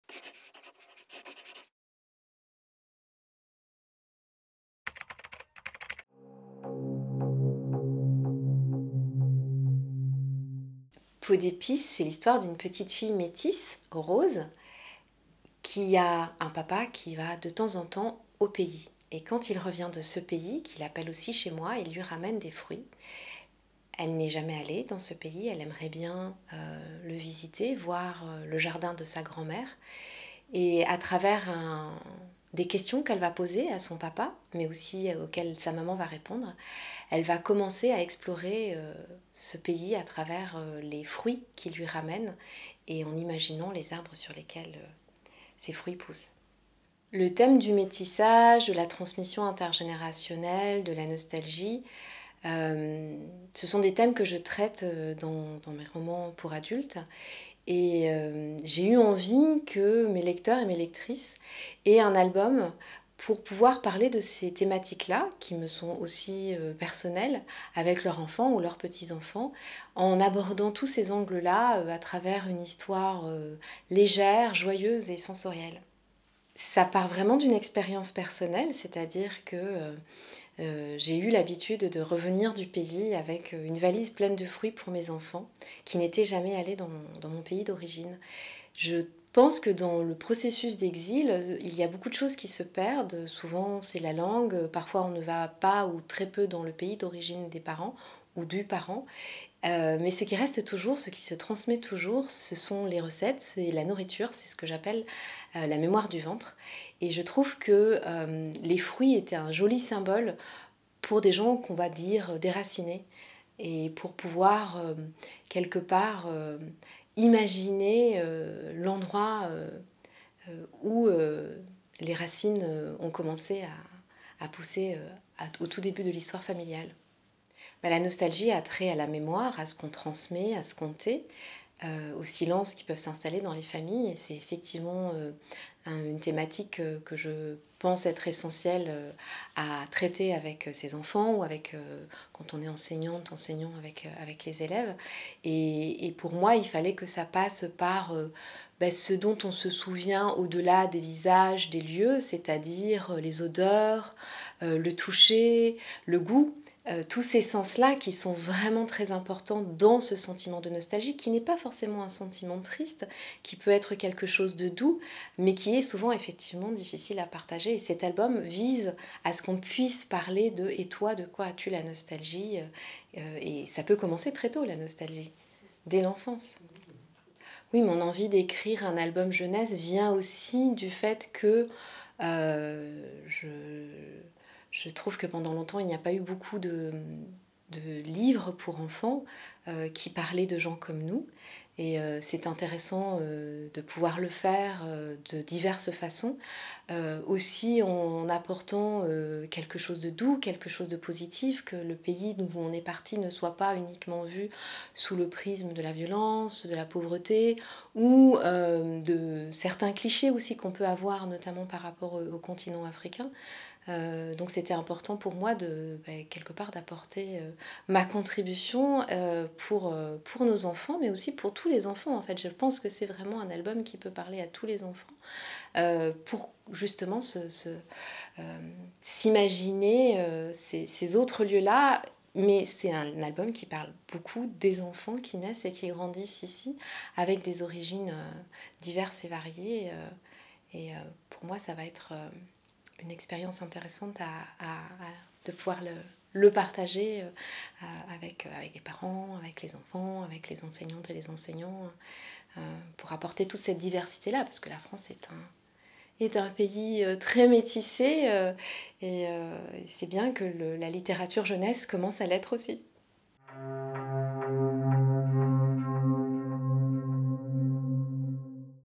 Beata Umubyeyi Mairesse - Peau d'épice
Beata Umubyeyi Mairesse vous présente son ouvrage "Peau d'épice". Parution le 21 septembre 2023 aux éditions Gallimard Jeunesse.